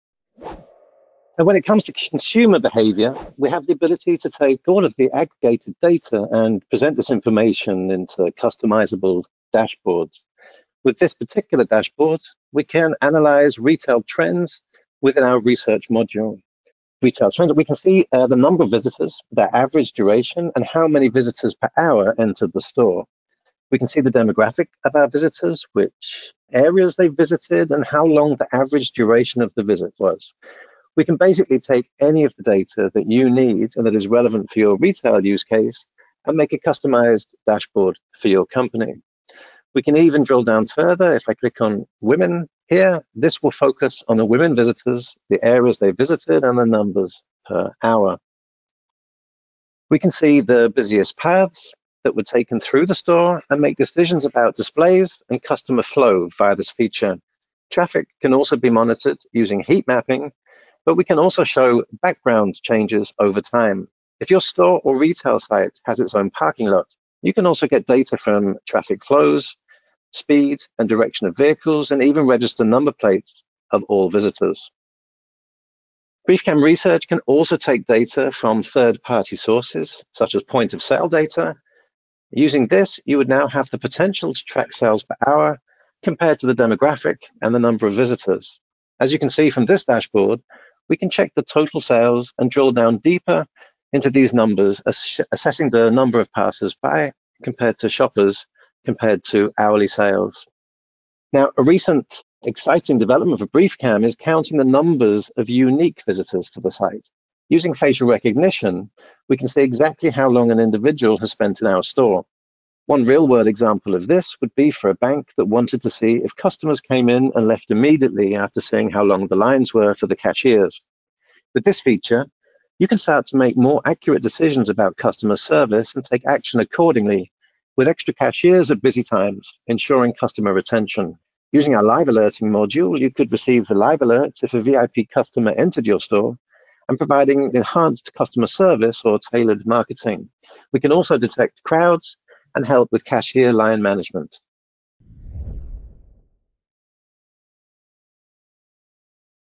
Video Analytics for Understanding Retail Consumer Behavior
Learn how the BriefCam Video Analytics Platform provides retailers not only with real-time alerts to be aware of queues and crowd size, but it also delivers crucial business intelligence in the form of dashboard reports. Those customizable visualizations can display key metrics such as unique customer traffic, bounce rates, demographics, volume, visit duration, as well as vehicle traffic and easily consumable heatmaps that indicate popular pathways for navigating a store and more. Retailers can also integrate BriefCam reporting with point-of-sale data, in order to correlate total sales with customer traffic and demographics trends.